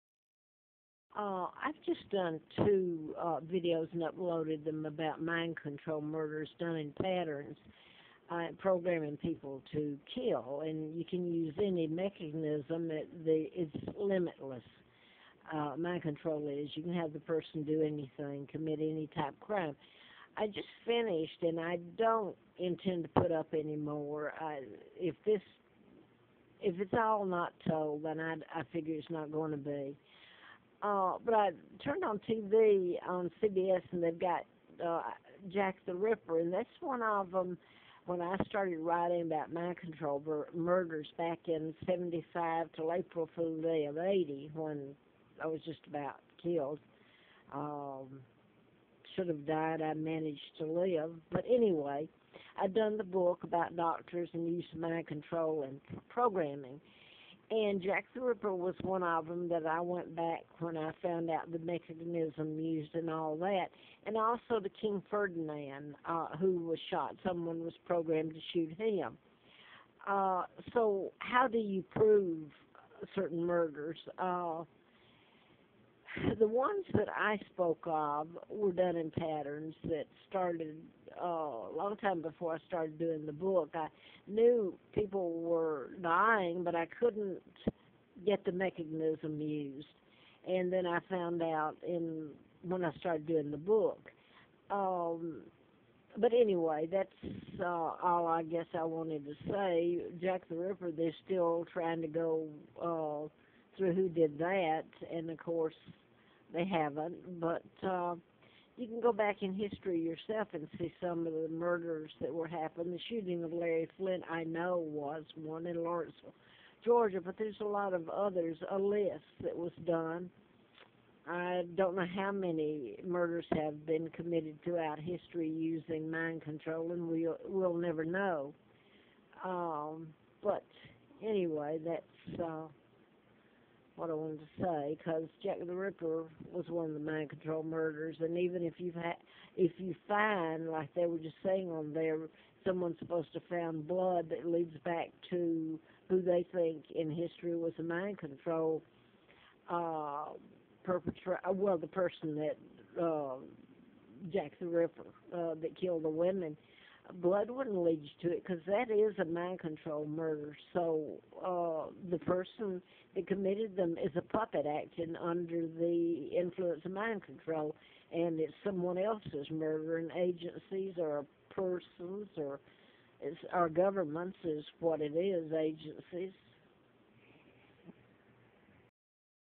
MINDCONTROL as MURDER WEAPON/ AS "DRONE"?
Drone can be used as murder weapon, powered unlimited, so is use of MINDCONTROL used influence one's decision making, program person(s) kill